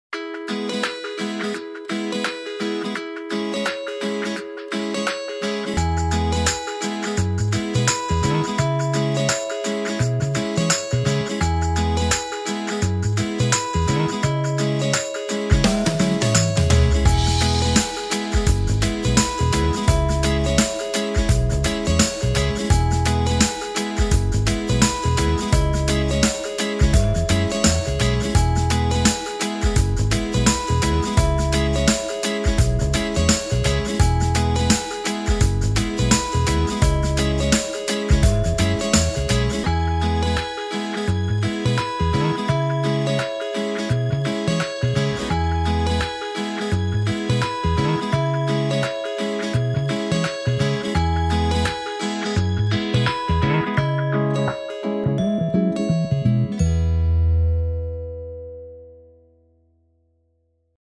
Tecniche di cottura: Scottatura senza grassi
Scopri in questo video le tecniche di cottura con le padelle antiaderenti.